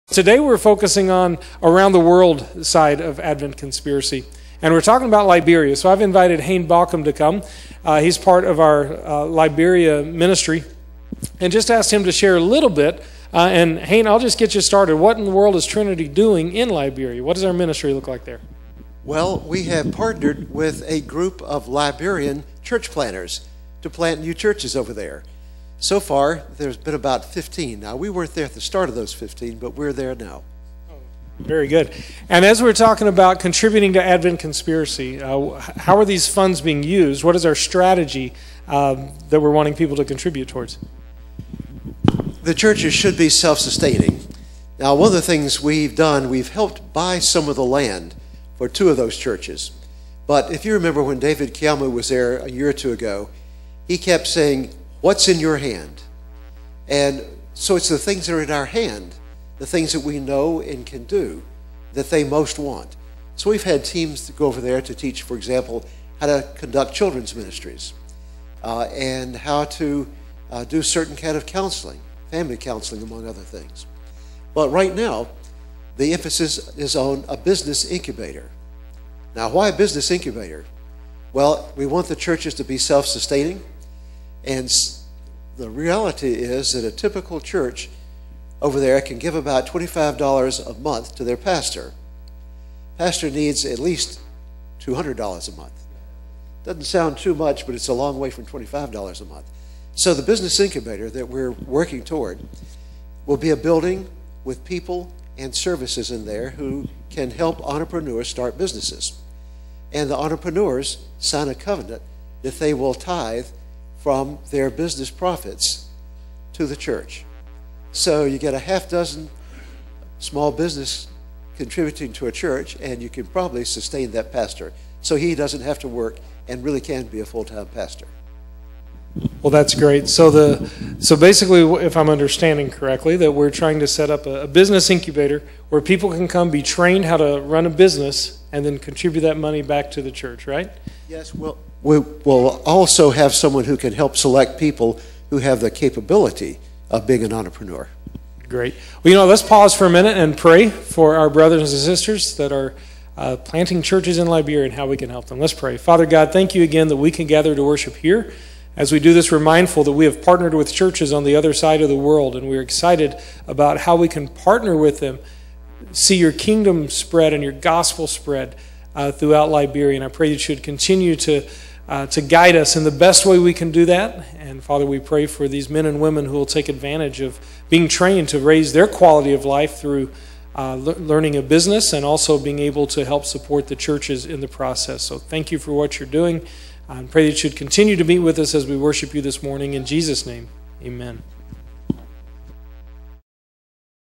Liberia Interview with Hayne Baucom
Hayne Baucom gives an update on our partnership with church planters in Liberia and how we can help by donating to Advent Conspiracy.